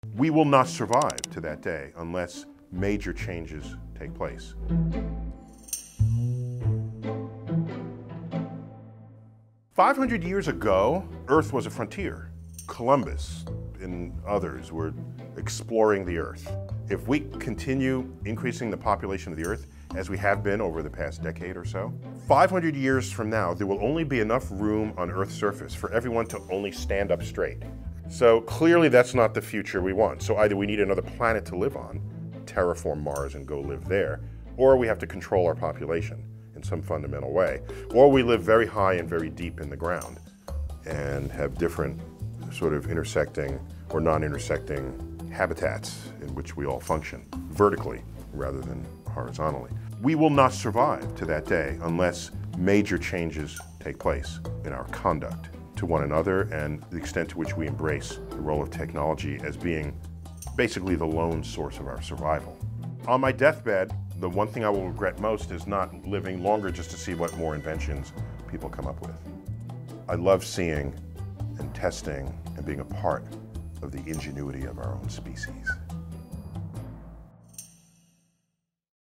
Neil deGrasse Tyson On The World In 500 Years
Neil deGrasse Tyson explains what the future looks like 500 years from now and how we need to embrace technology.

Voices of Color explores the issues that matter to minority groups and communities through politics, education, entertainment, and all other aspects of culture.